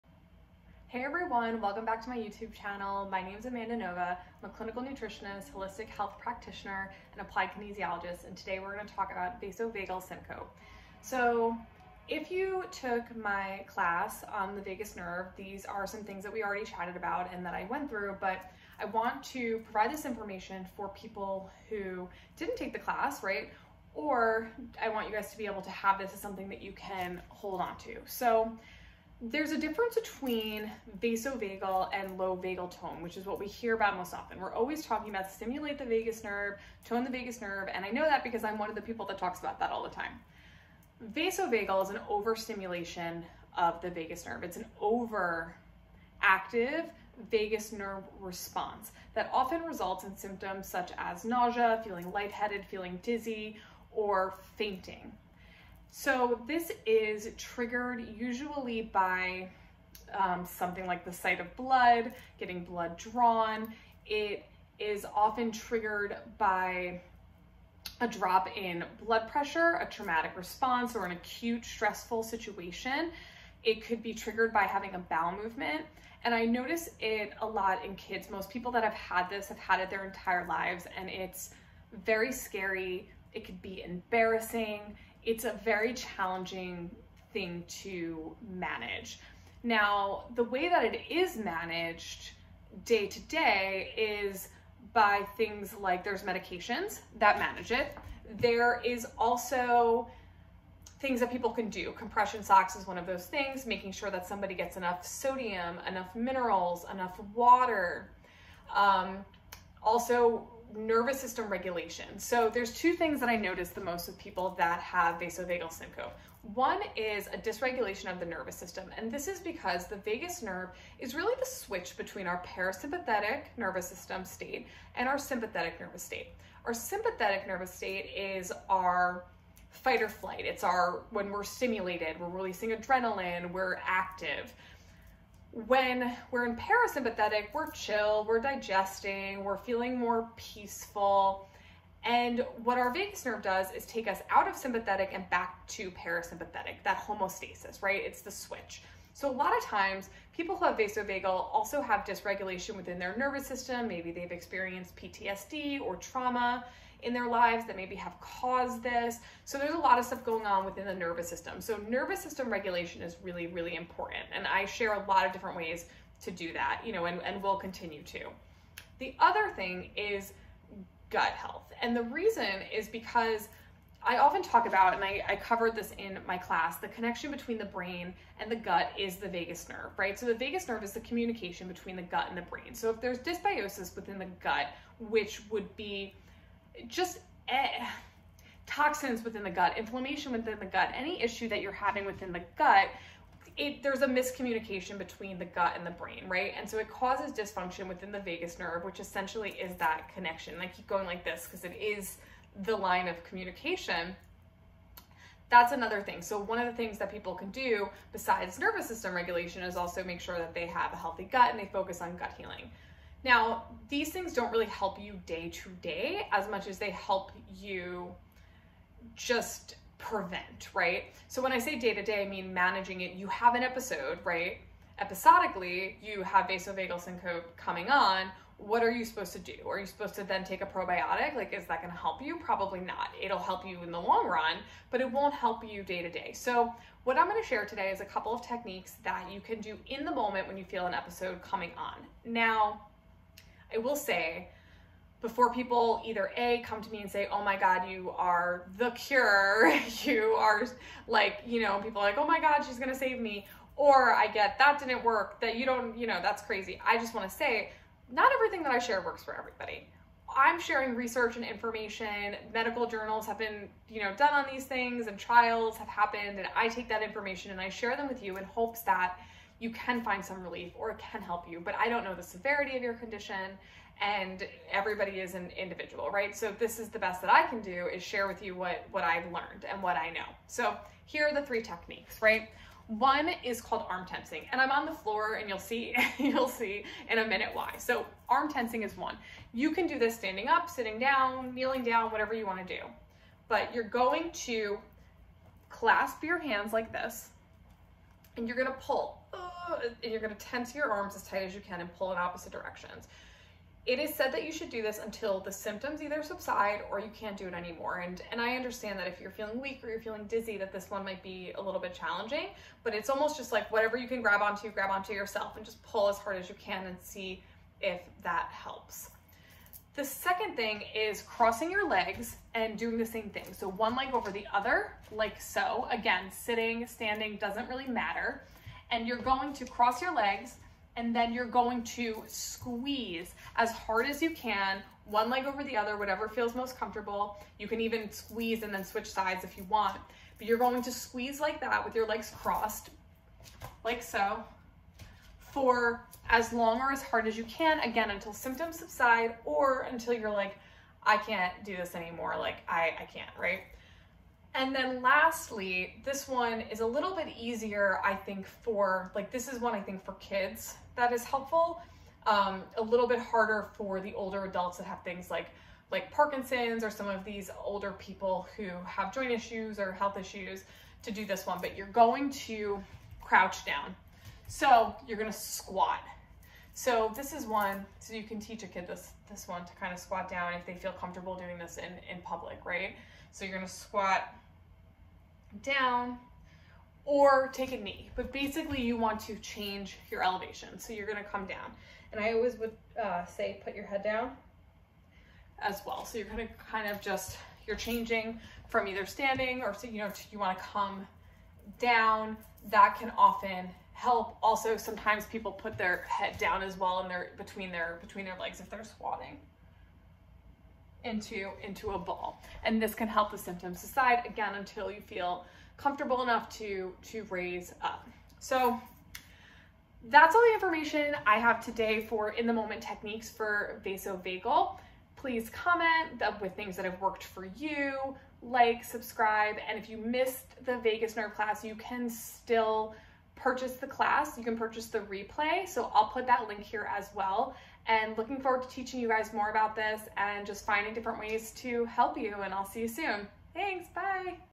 Counter Techniques for Vasovagal Syncope
If you have been following me on other social media platforms, you know that I posted a reel on vasovagal syncope and listed some support that is often used to treat syncope for prevention management of it. The techniques in this episode today are more episodic techniques that have been shown to be effective when someone feels vasovagal syncope coming on.  I just want to make it clear that I am just sharing what I have learned and know in case this may help someone who needs it - this may or may not work for everyone as all people and cases are different!

If you missed my class on the vagus nerve, here is a link to grab the replay